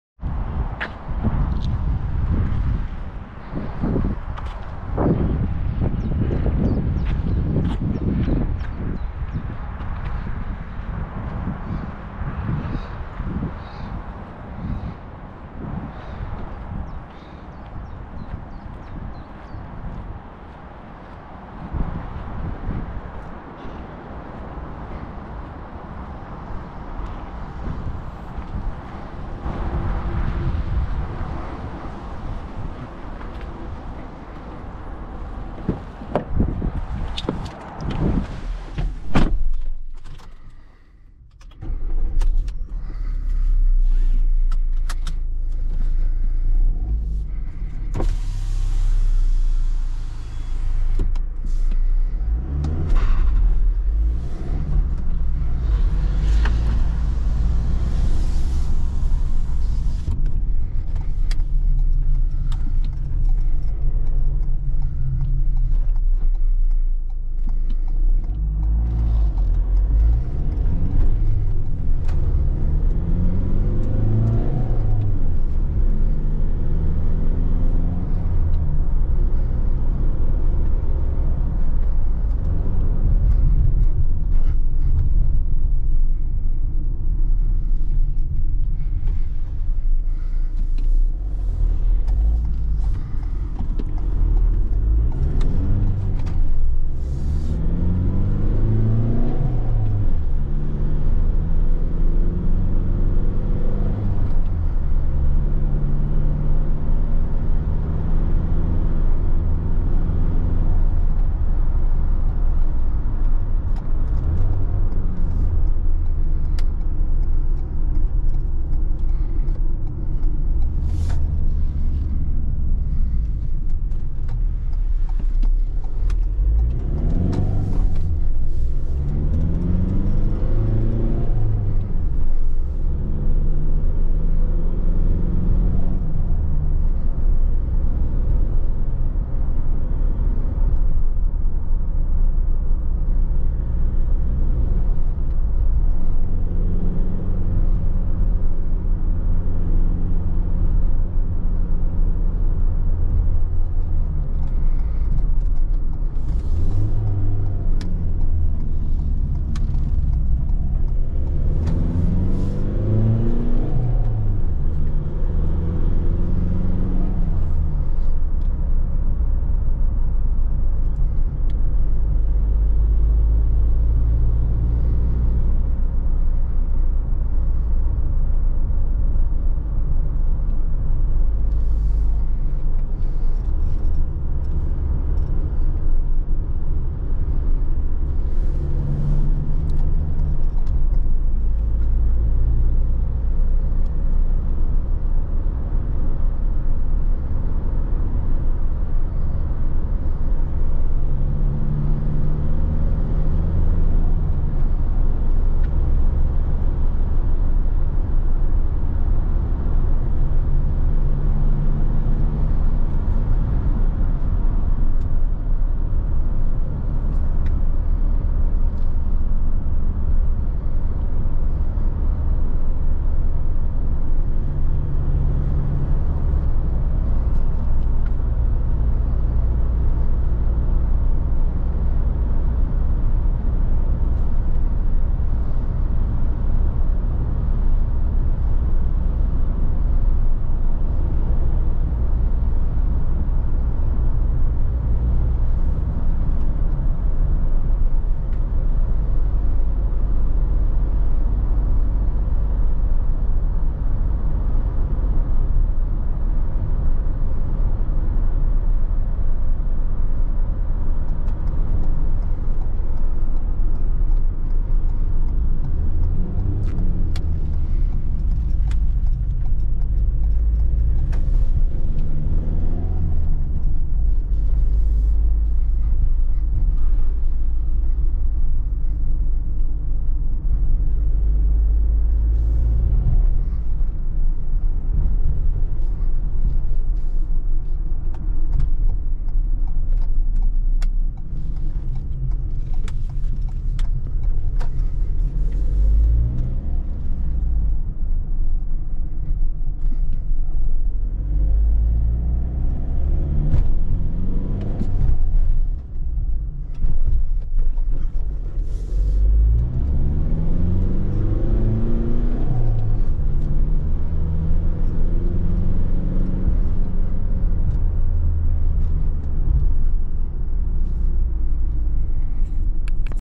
2023 Ford Transit Custom | 2.0 TDCI 130HP | POV Test Drive
Car: Ford Transit Custom L1H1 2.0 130HP 2023

Engine: 2.0L Diesel 4-cylinders
Displacement (cc): 2000
Power: 96 kW/130 hp
Torque: 385 Nm
Transmission: 6-speed manual
Traction: FWD ( Front-Wheel Drive )